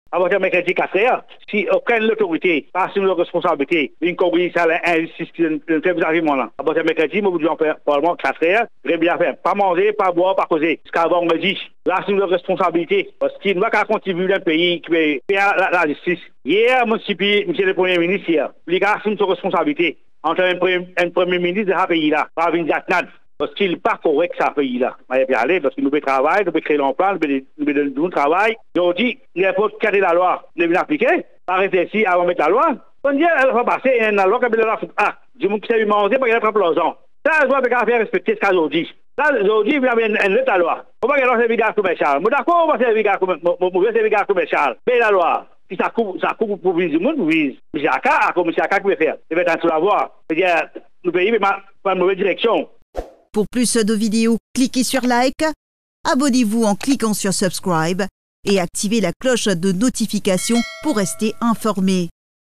Roti Aka menace d’entamer une grève de la faim à partir de ce mercredi
Il dénonce le prix de la bonbonne de gaz à Rs 1 000 l’unité pour les commerces : Roti Aka menace d’entamer une grève de la faim à partir de ce mercredi